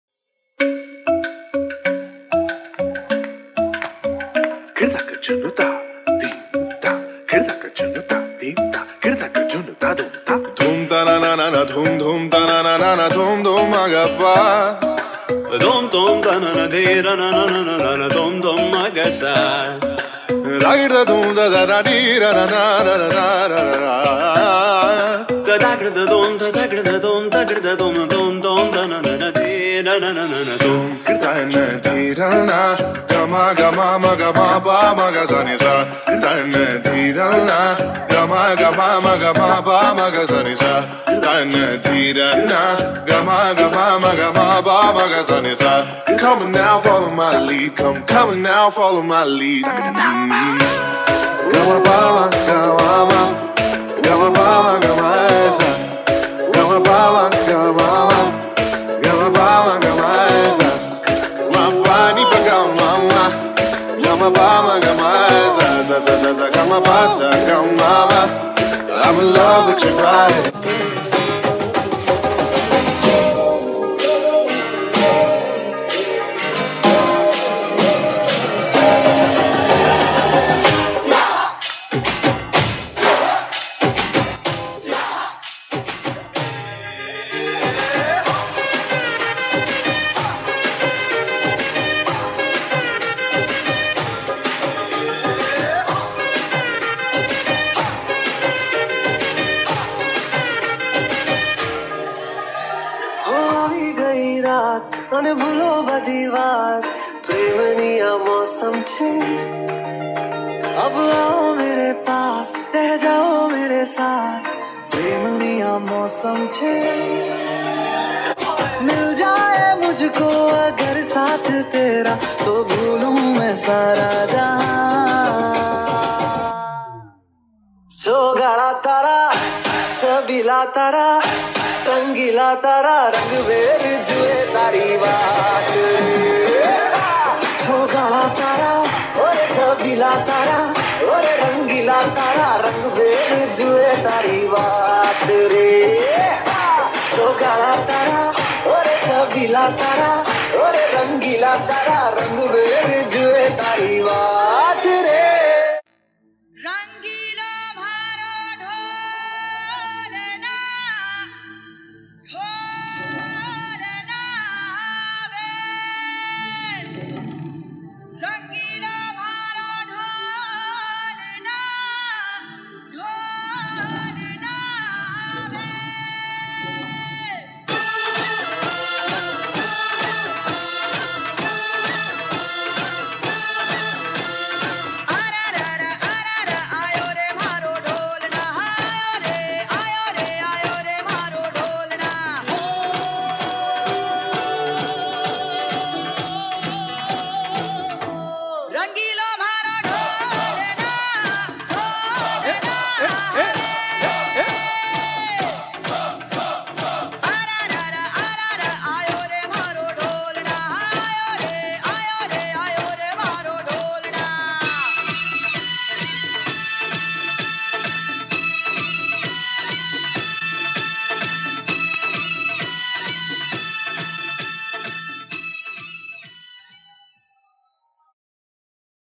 SGKC | Onam 2023 | Onam Celebration | Fusion Dance by Martha Mariam Vanitha Samajam
Fusion Dance displayed by Martha Mariam Vanitha Samajam in Onam Celebration 2023 - Onam program conducted by St.George Knanaya Church, Dubai.